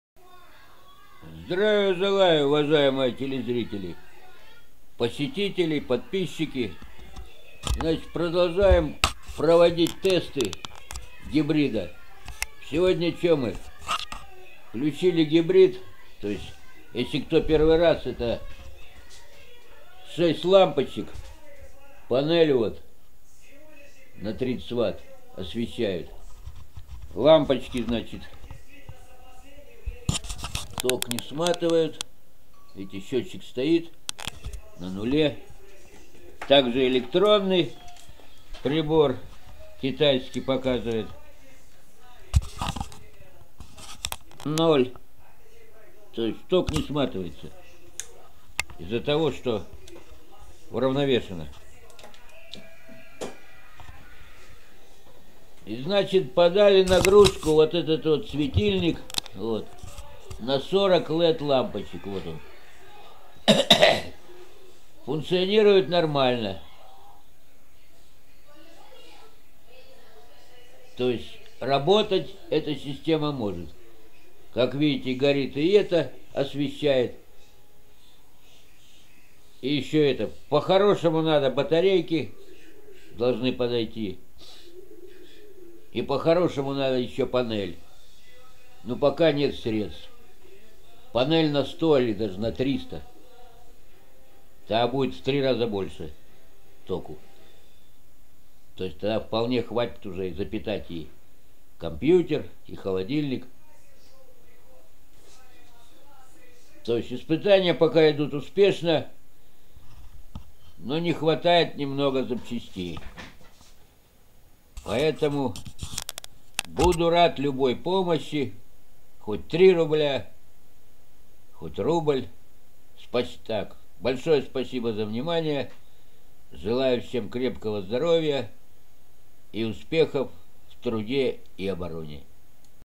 Без топливный генератор Гибрид на солнечной панели испытания
Продолжаем испытания пока работает успешно